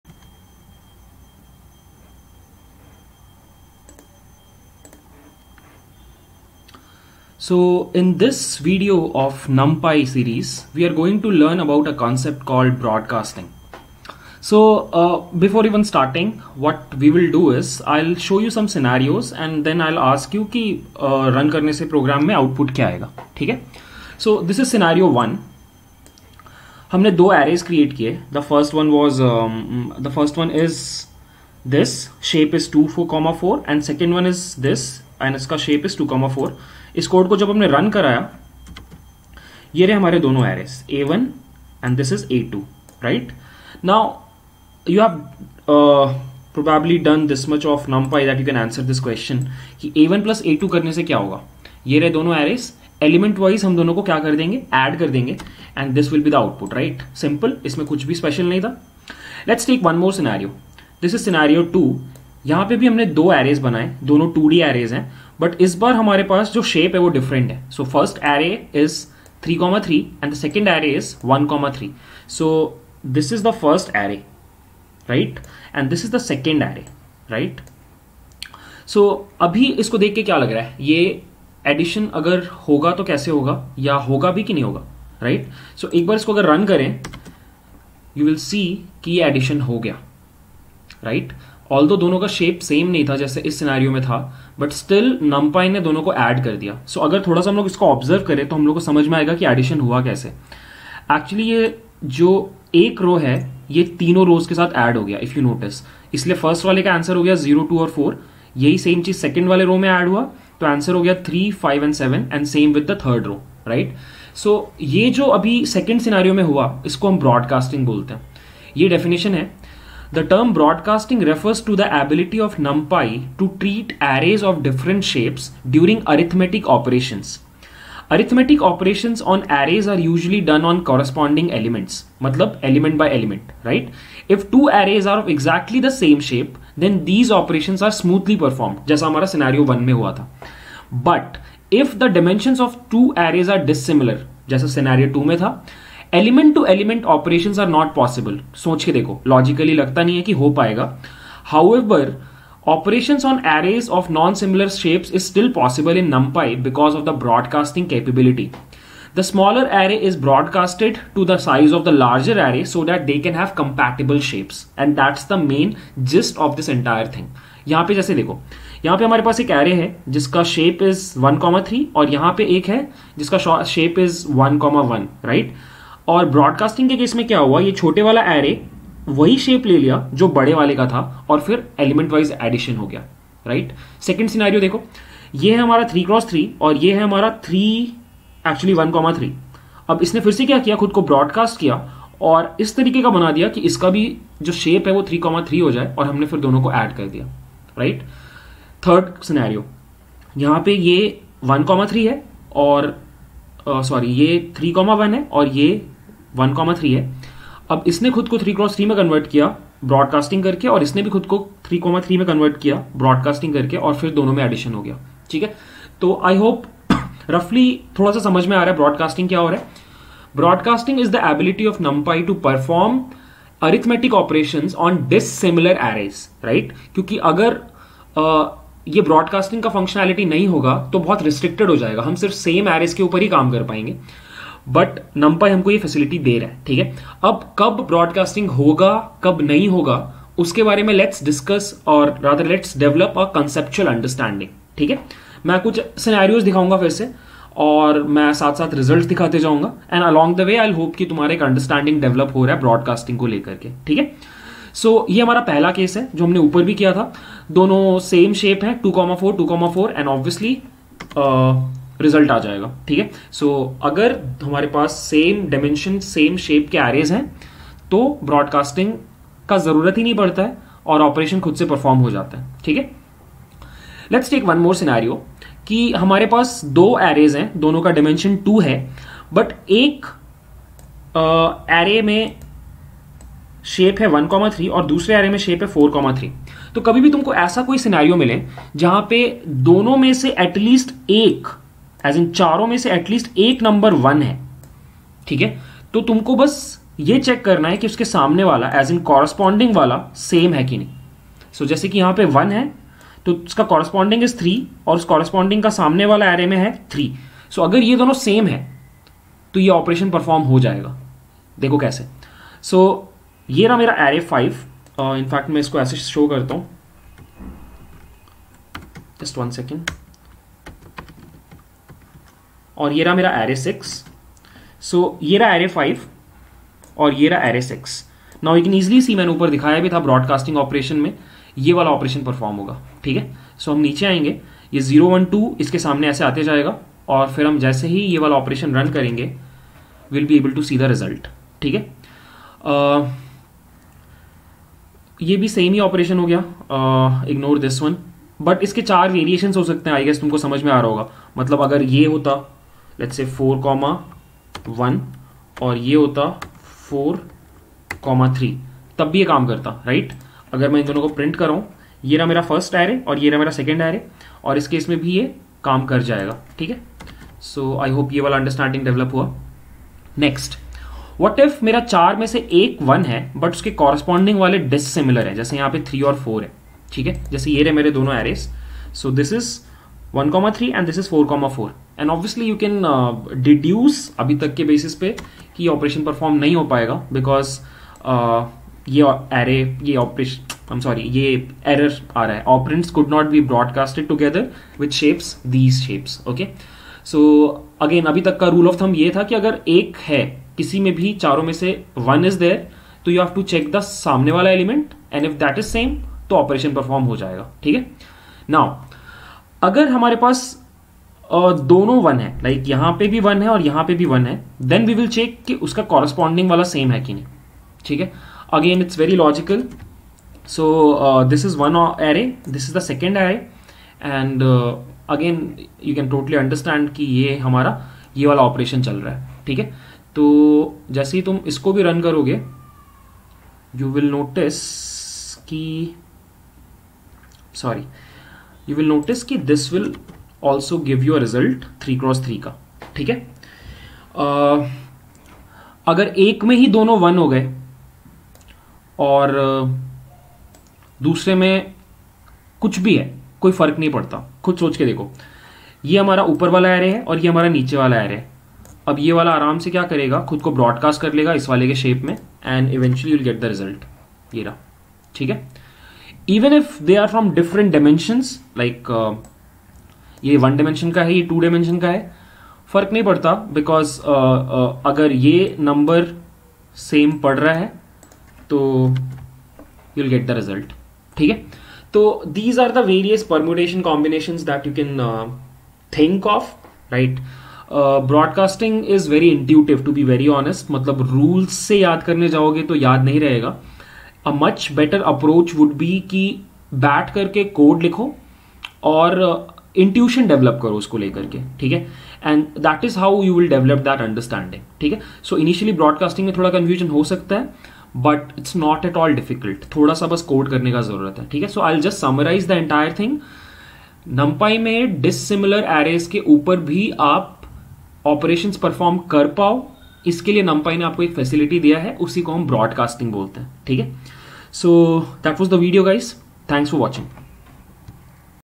Numpy Part 12 - Broadcasting
In this video, we will understand a very important numpy concept known as broadcasting. About CampusX: